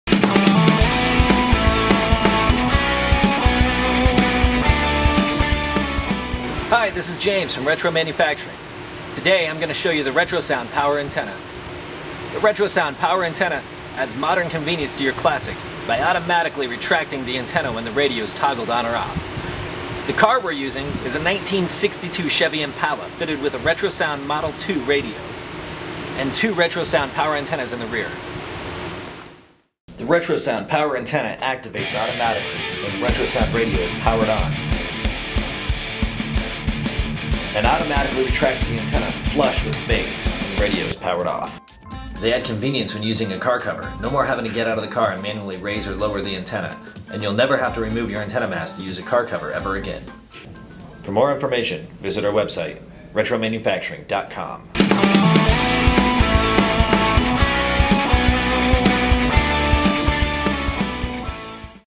RetroSound Power Antenna Promo
The RetroSound power antennas add modern convenience to your classic by automatically retracting the antenna when the radio is toggled on or off.